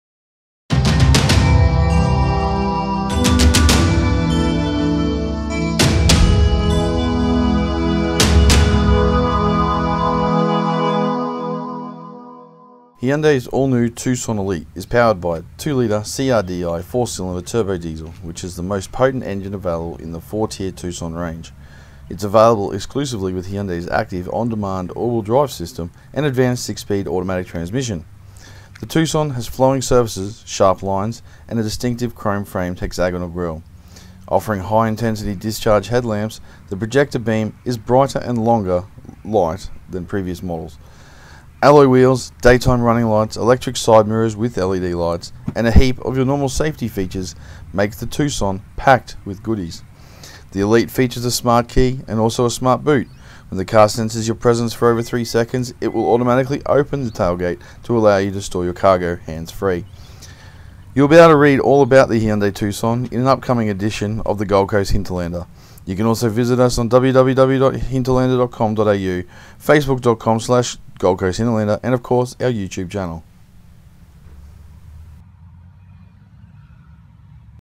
2015 Hyundai Tucson Elite
Small intro into the 2015 Hyundai Tucson Elite supplied by Hyundai Australia.
You can read other reviews and lots of local stories and of course you can like our Facebook